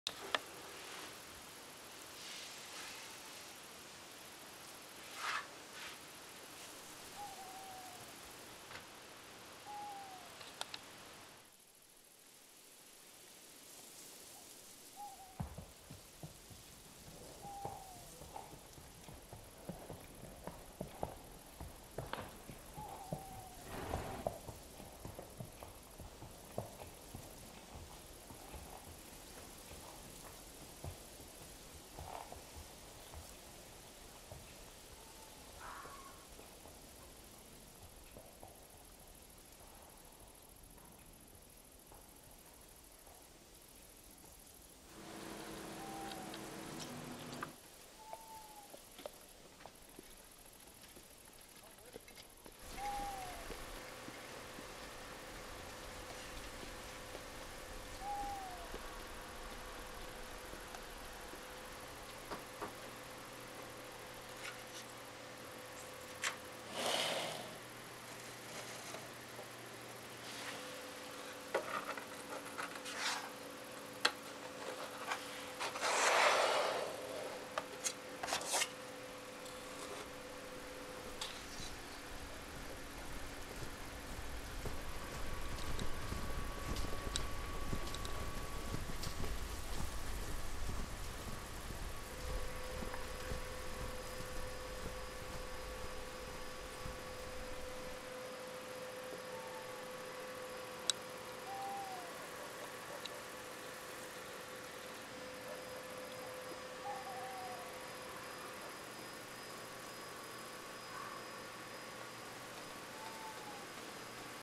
1961 Dodge D100 build announcement
♦ 1/6 scale ♦

The frame for the cabin is printed from polyamide.
Body parts will be built of styrene.

3d modeling software I use is Blender.